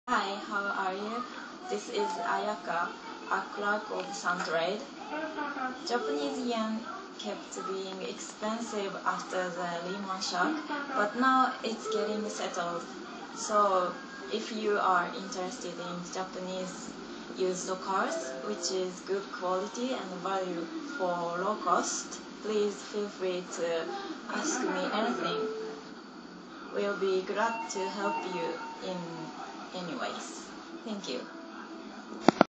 Japanese used car exporter SUN TRADE greetings
is our website. I'm sales assistant of SunTrade in Matsuyama branch